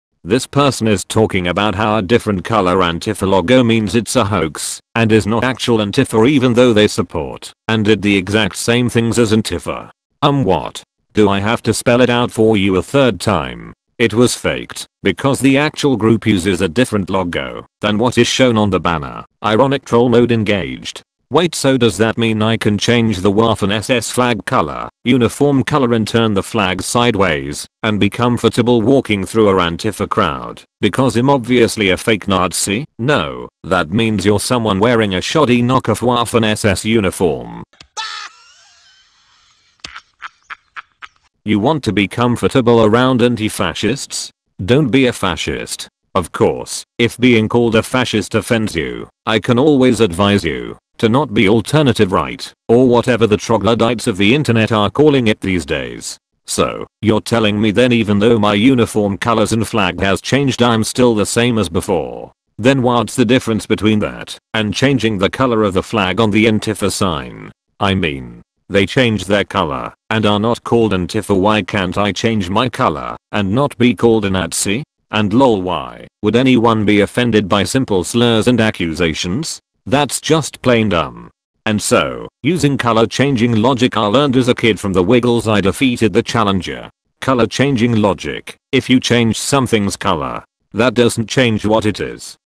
How To Correctly Lose An Argument Part 2: Faulty Logic
She never responded back after that...

Unlike the last video this has context! (might remake part 1 later)
Although the text might be moving a bit fast... oh well ill fix that next video